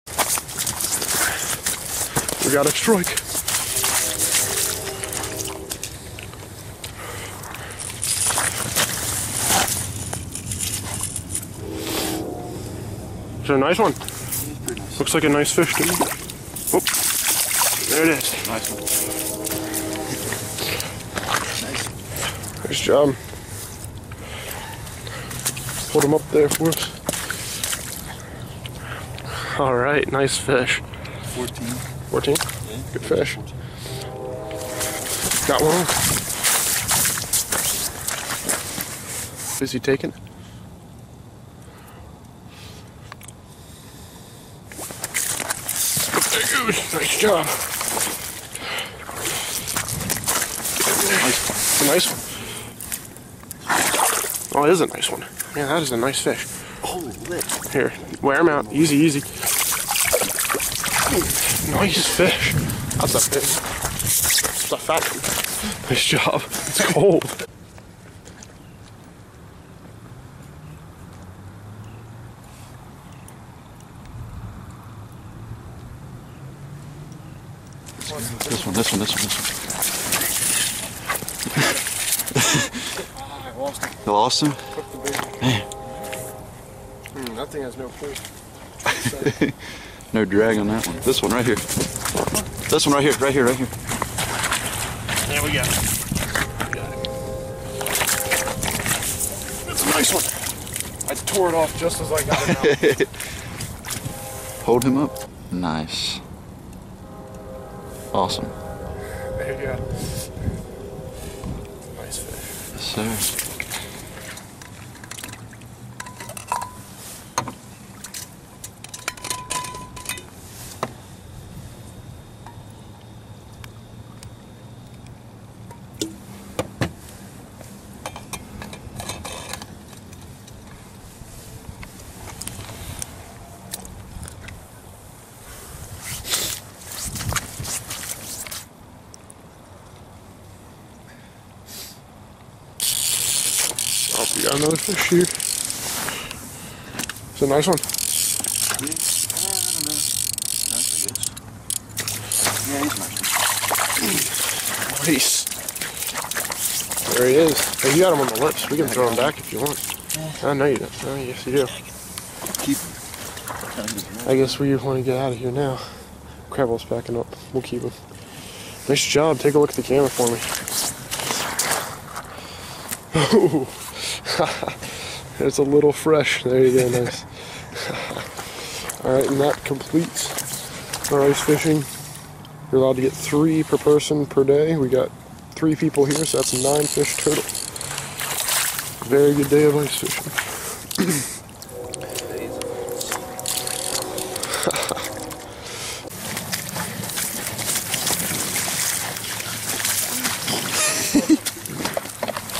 Ice Fishing Pennsylvania 2014
Brandon and Herschel head to Lebanon County, Pennsylvania to go ice fishing for trout in winter 2014.